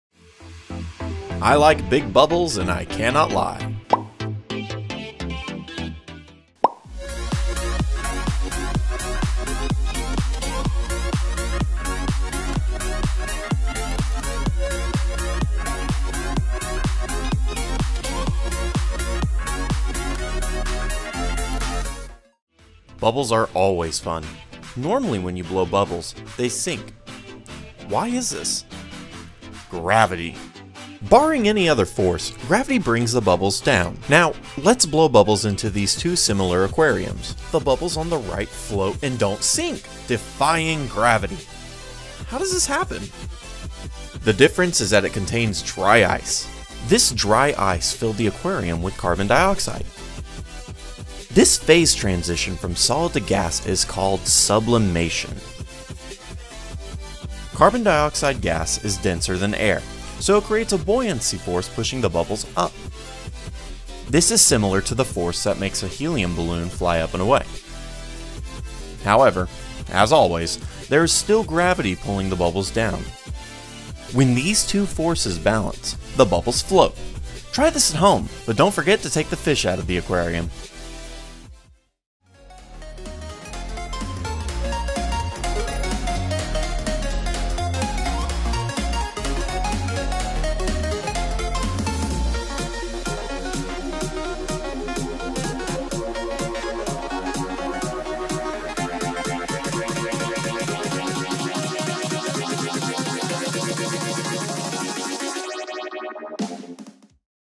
Magic Bubbles
Like to blow bubbles? Tired of seeing them fall to the ground and pop? Wingardium Physics-osa, come see how to make bubbles float in the real world!